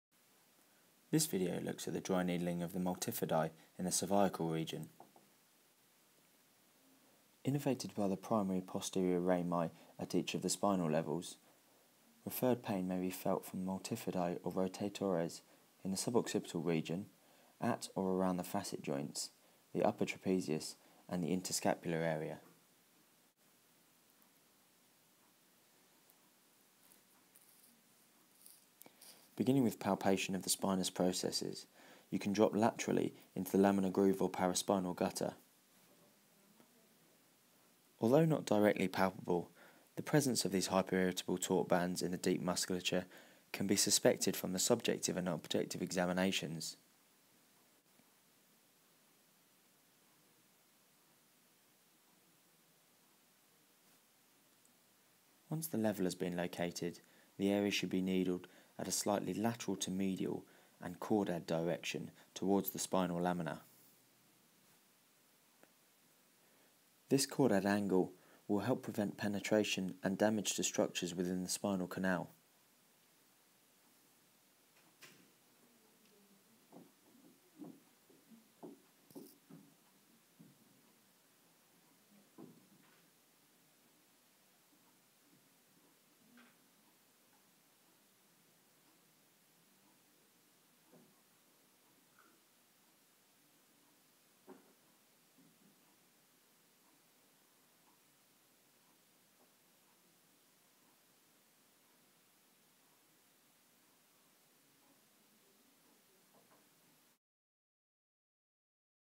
Dry Needling: Cervical Multifidi
This video looks at a dry needling technique for the cervical multifidi.

Please do not attempt dry needling unless you are suitable trained and qualified to do so.

Comments and discussion welcome.